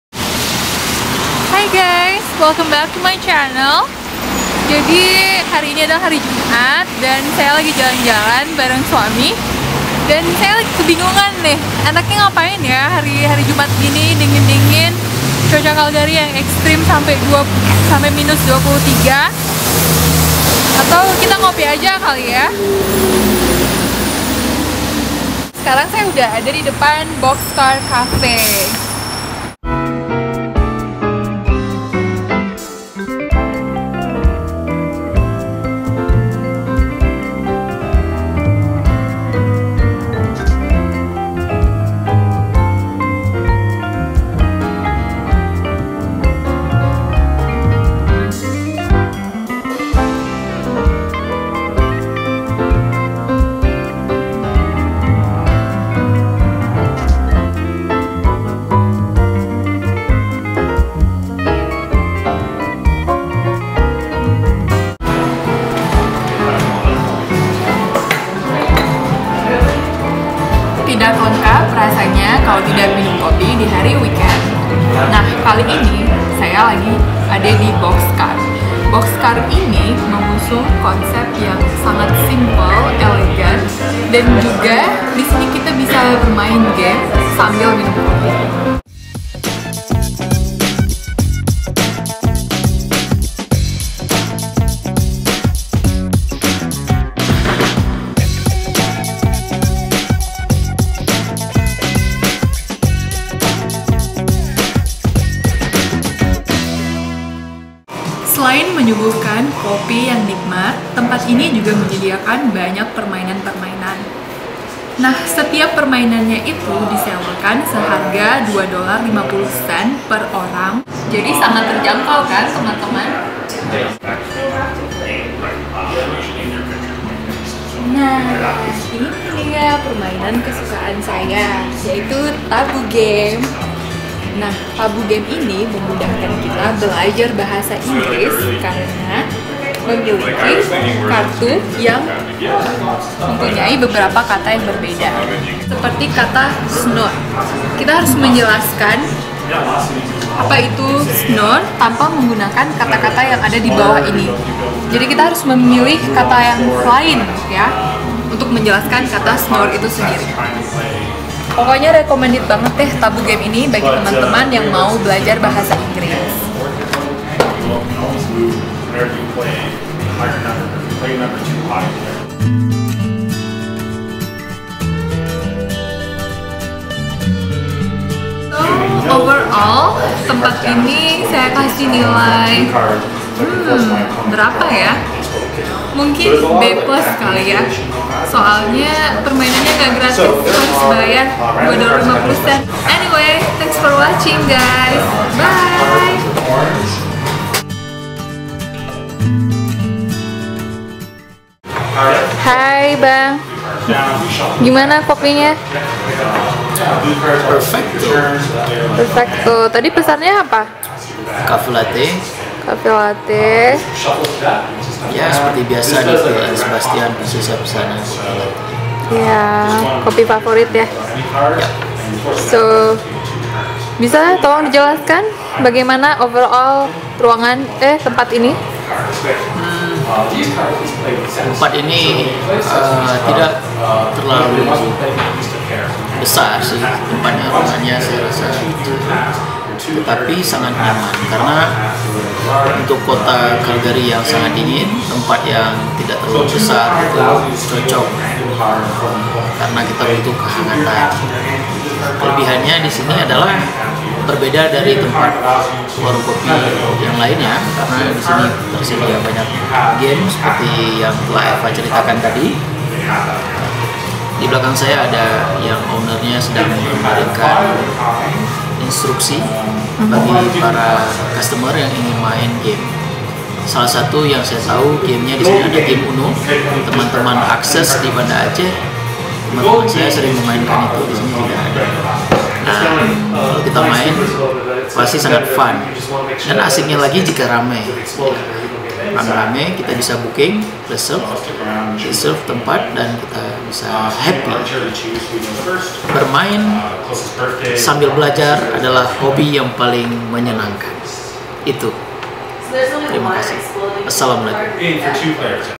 Ngopi sambil main game
Bagi teman-teman yang sedang berada di Calgary, Canada, jangan lupa singgah di BoxCar Cafe. Di jamin bakalan fun!

I'm sorry for my poor quality video! Masih belajar editting! Enjoy :D